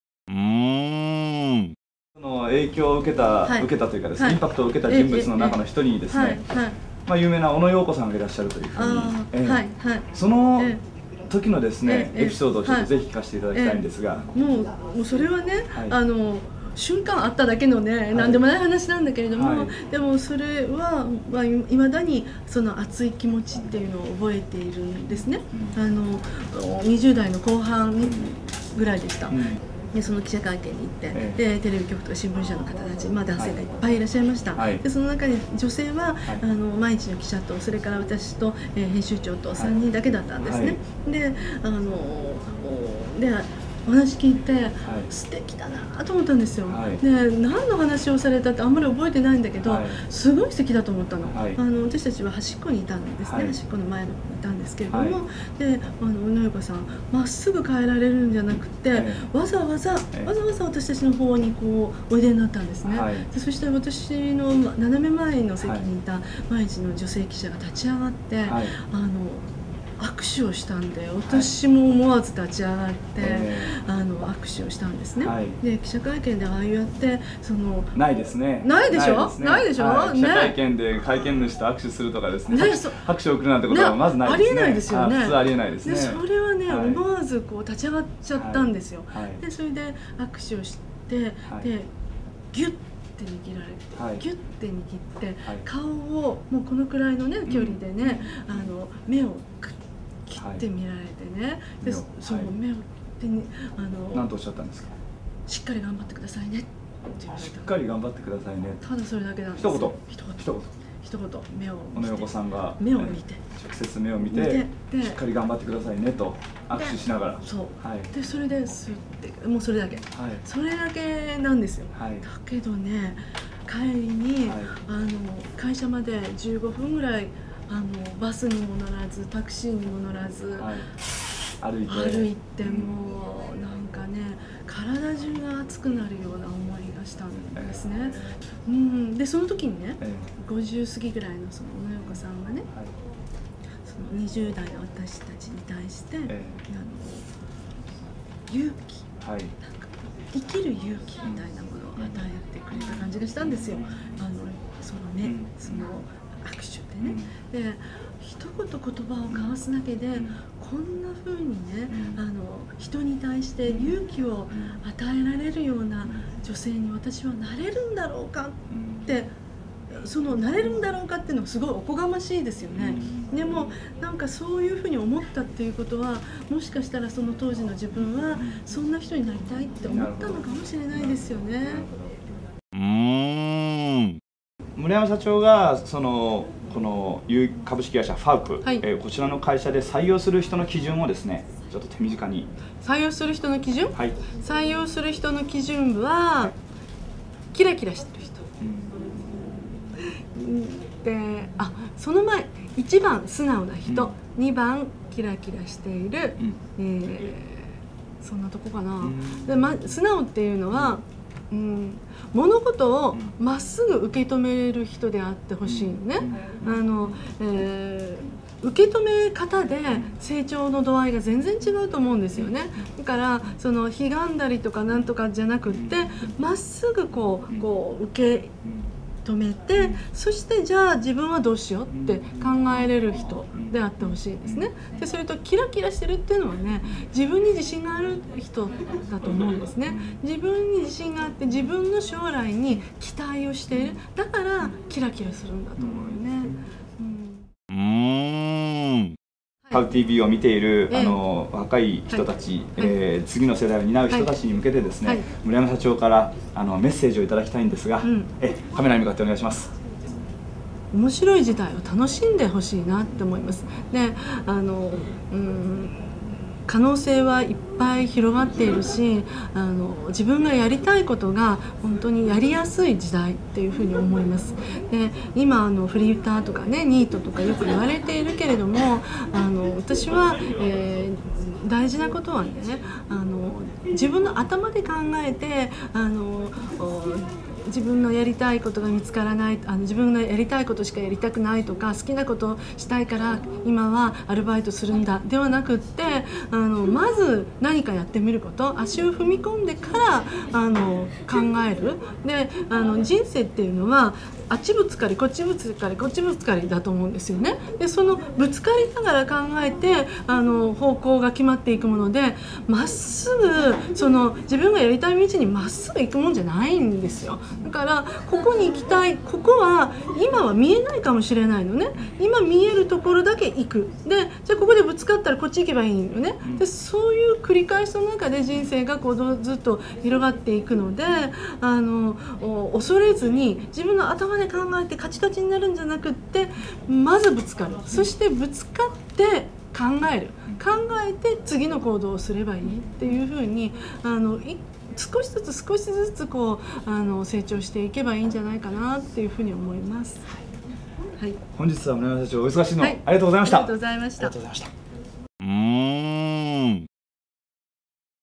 【アヴァンティ（4）】オノ・ヨーコとの出会い、衝撃！
「オノ・ヨーコとの出会い、衝撃！」
■取材協力：株式会社アヴァンティ   創業者　村山由香里さん
■事業内容：「アヴァンティ」の編集・出版イベント事業

▼シリーズ
・働く女性を応援します（1/4）
・このままじゃ死ぬ！アヴァンティ誕生秘話（2/4）
・福岡の女はバカじゃない！アヴァンティの志（3/4）